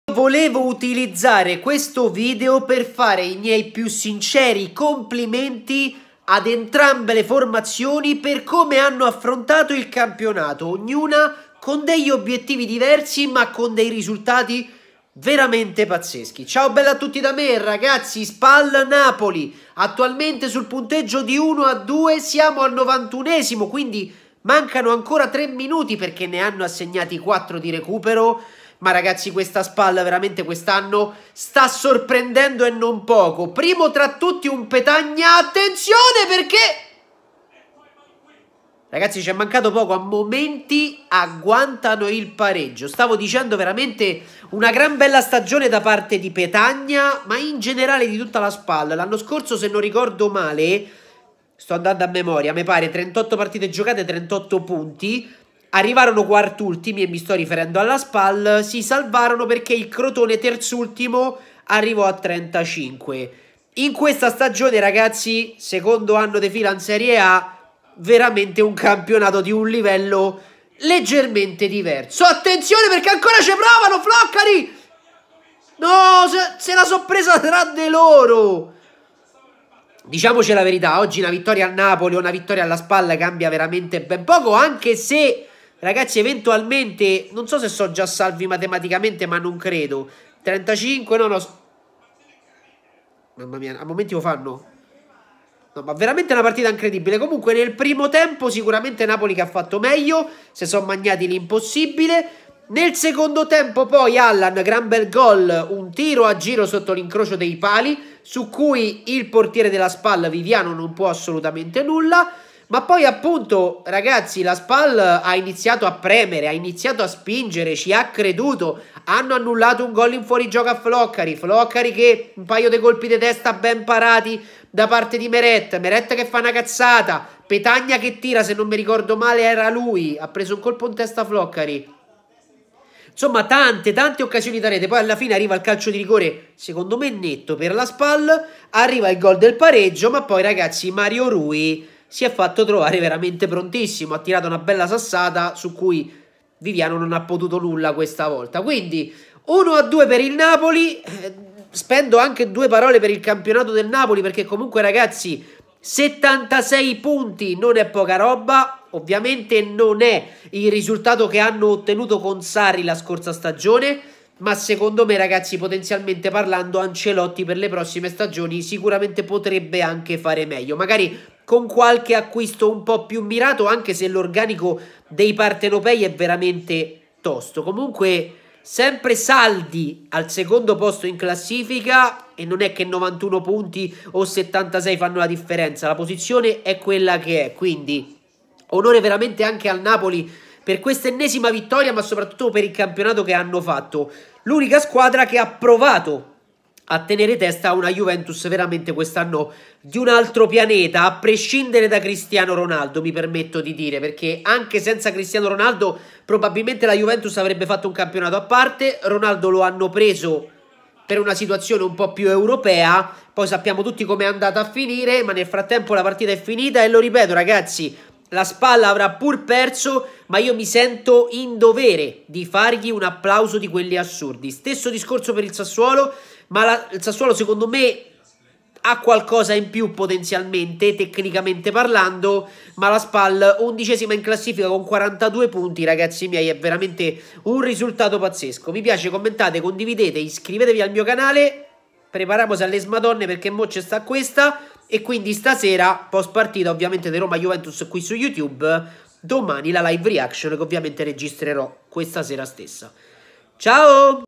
SOLO APPLAUSI!!! UN CAMPIONATO PAZZESCO PER ENTRAMBE!!! 👏🏻👏🏻👏🏻 Spal-Napoli 1-2
Spal-Napoli 1-2: complimenti ad entrambe per il super campionato!!! Serie A